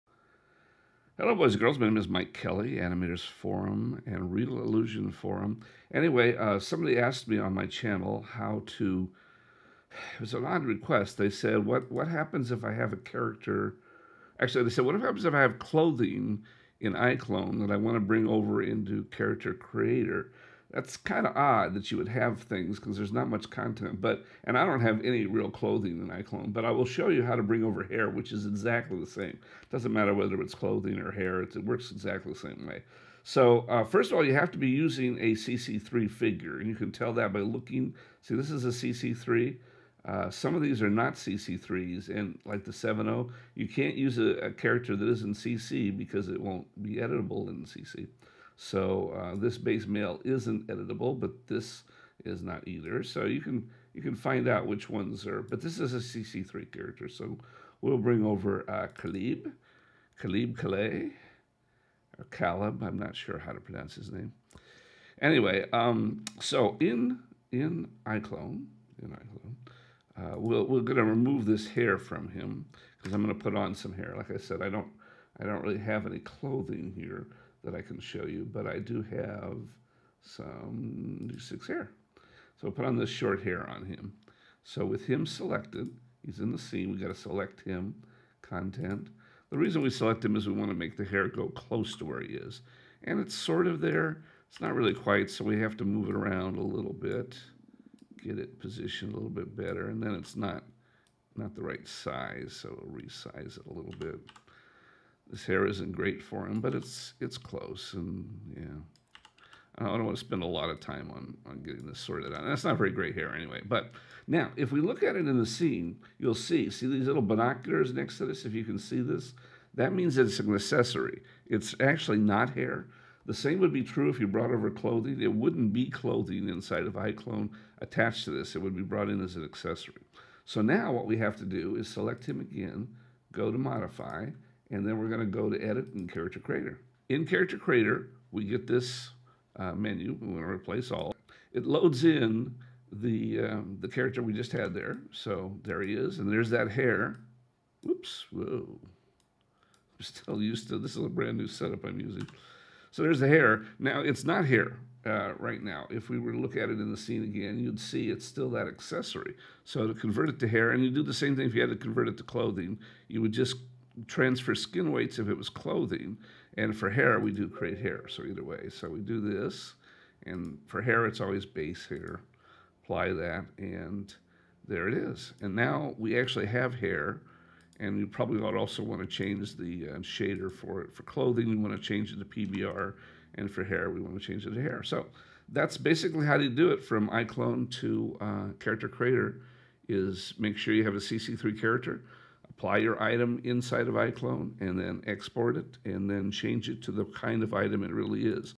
Export items from iClone to CC3
Showing how to take items inside of iClone and bring them into CC3 (yeah, I know, pretty weird but a request).